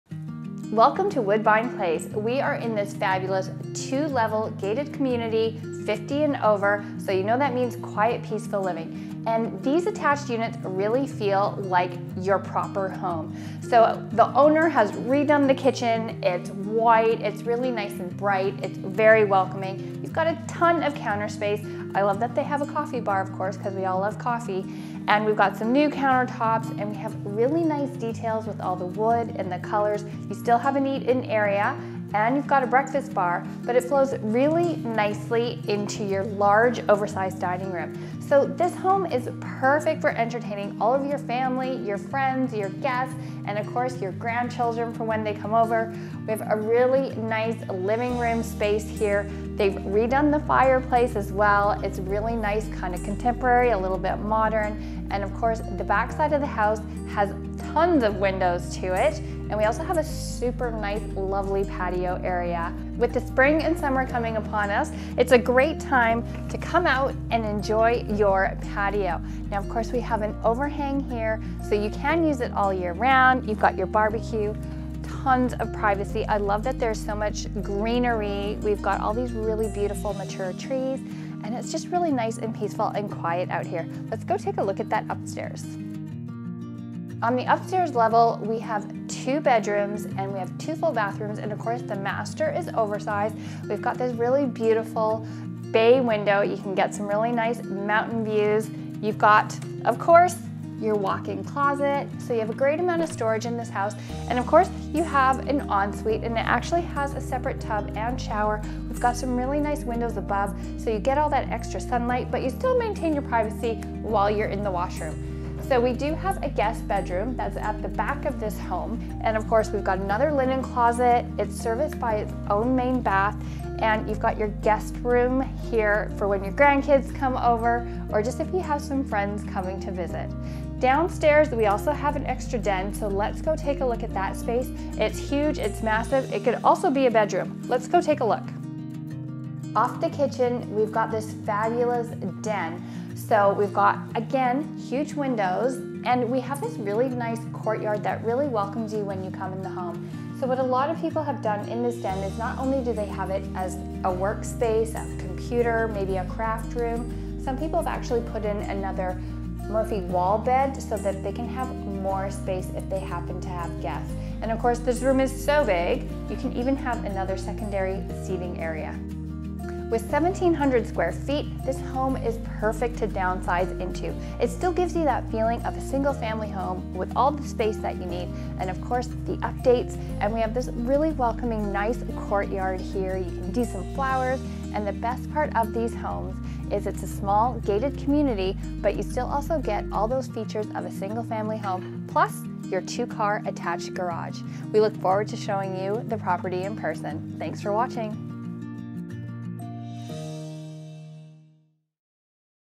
SOLD! | 14 - 9515 Woodbine Street Chilliwack BC Long Version
Welcome to this well maintained, move in ready 2 level home nicely tucked away making for quiet and peaceful living close to shopping, transit and District 1881. These homes have a great use of interior and exterior space plus welcoming courtyard plus a luscious green backyard space with your covered patio.

Bright remodeled eat-in kitchen, modern cabinets, stainless steel appliances, modern paint colors, dining room with board & batten, re-freshed gas fireplace, 12' vaulted ceiling in living room, large windows all long the back allowing tons of sunlight and open 2 story welcoming foyer.

The den on the main makes an excellent 3rd bedroom, office or craft area. All rooms are oversized providing easy living plus an attached 2 car garage.

Make this 55+ gated community your new home!

MLS R2779737
Asking $529,900
1,674 sq ft
1 pet allowed | 55+ age restricted